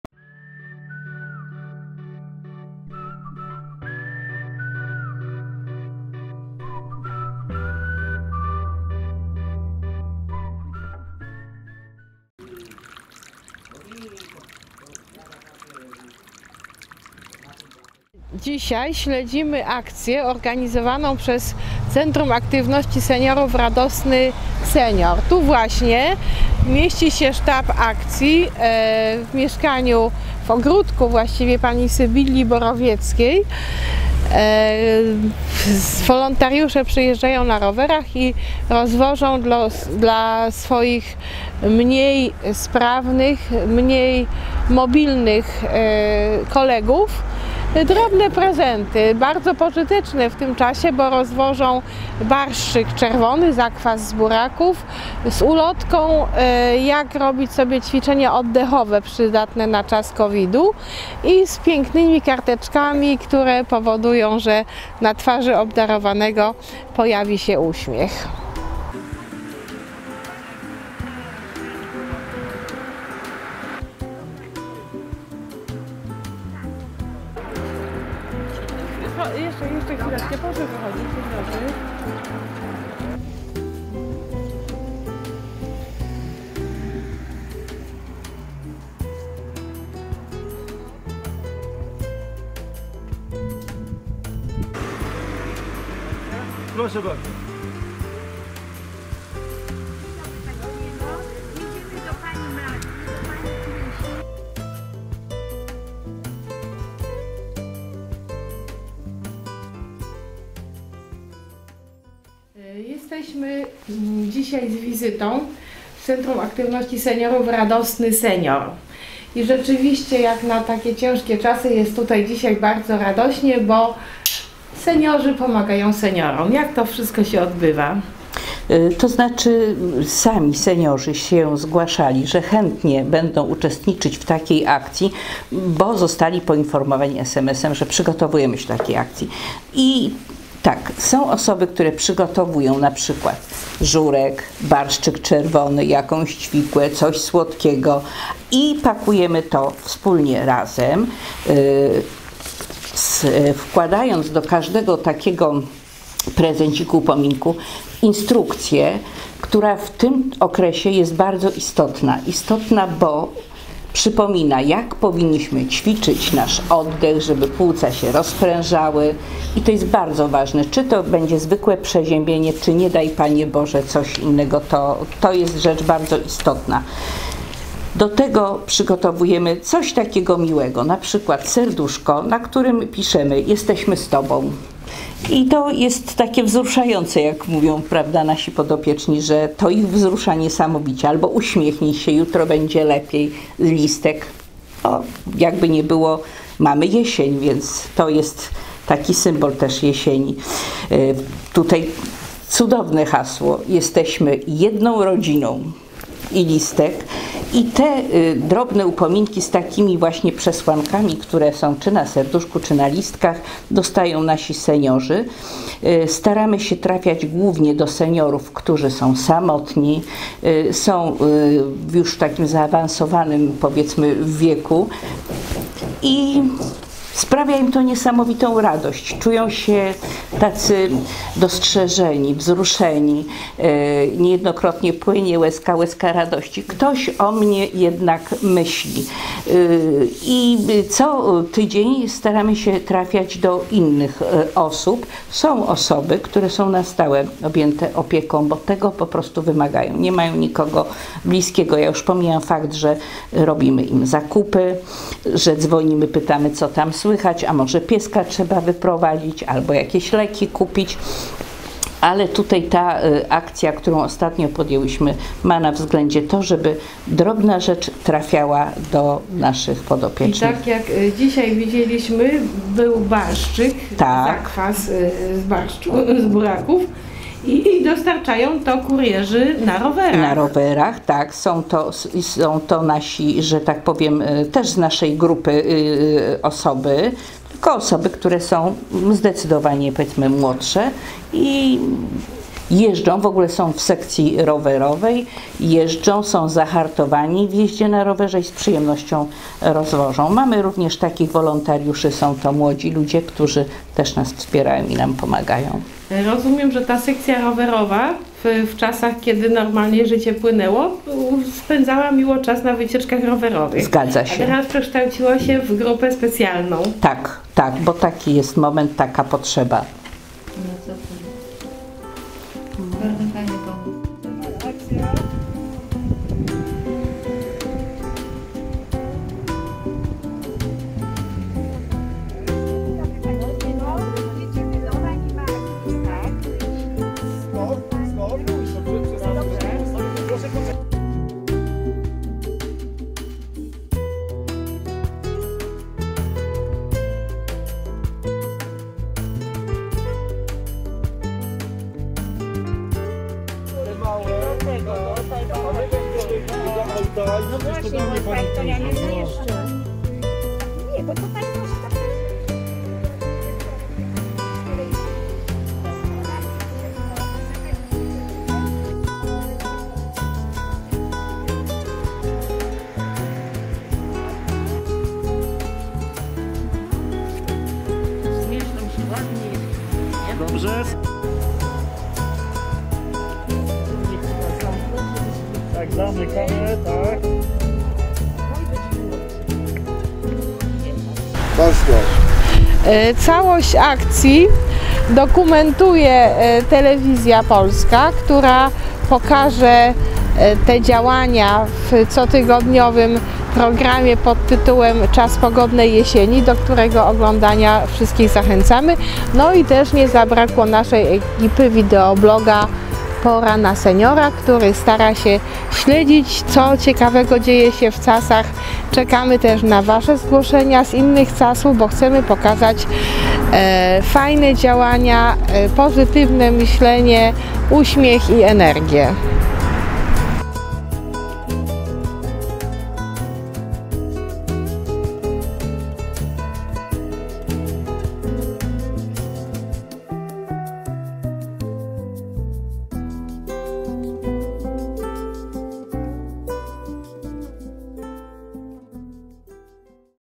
Radosna akcja w Radosnym Seniorze
Obdarowywać innych - sama radość. Sprawić ją można prostym upominkiem - dowodem pamięci i serdeczności. Barszczyk z zakwasu buraczanego z miłą dedykacją przesyłany Seniorom przez Seniorów z CAS Radosny Senior na pewno przywoła uśmiech i pokrzepi. Spróbujcie takich akcji na własnym podwórku, bo cóż możemy zrobić lepszego jak cieszyć się wspólną radością !